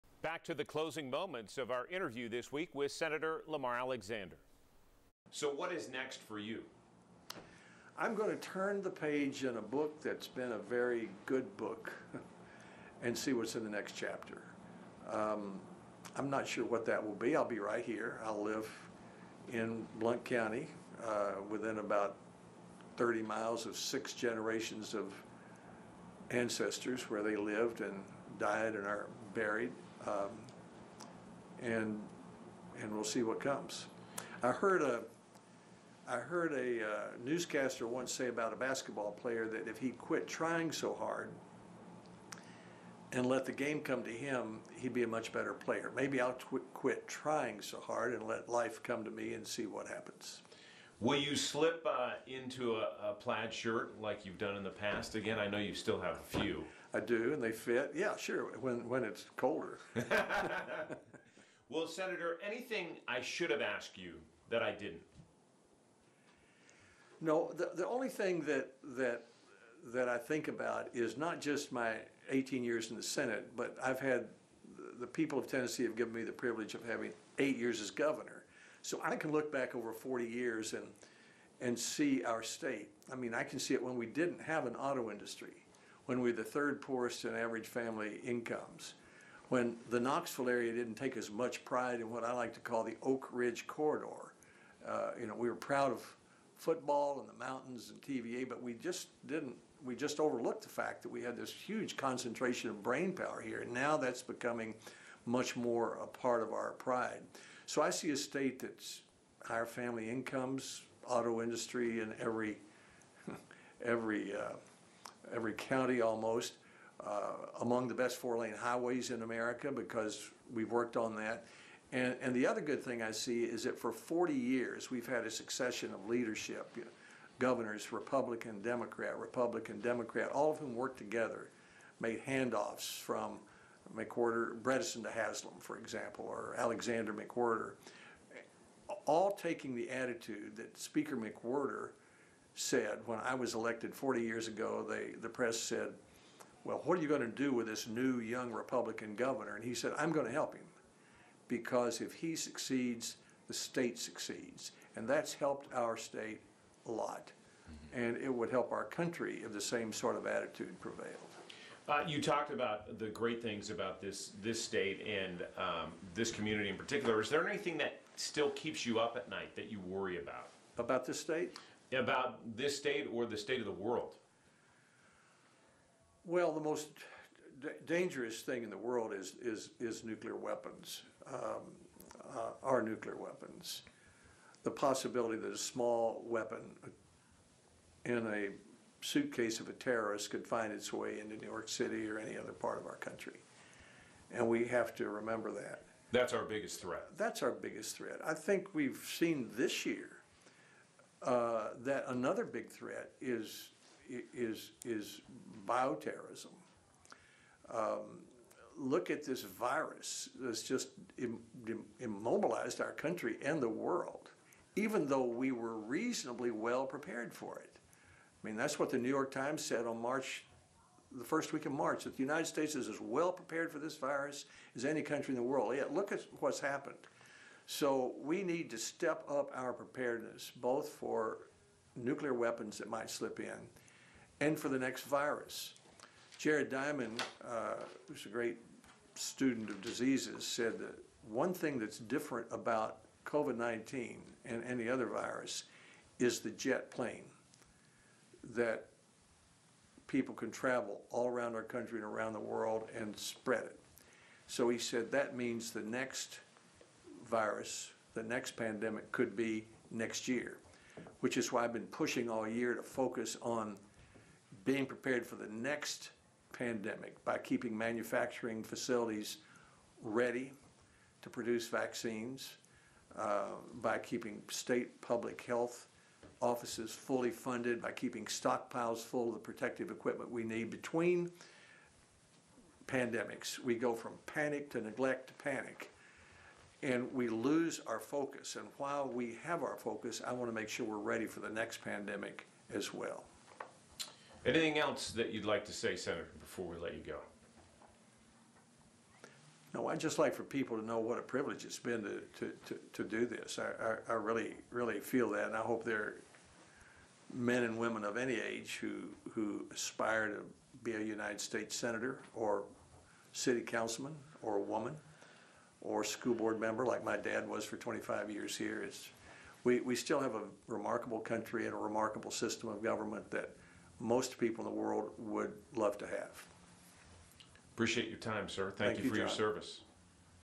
Lamar Alexander talks about retirement
Retiring Sen. Lamar Alexander talks about his time in public service.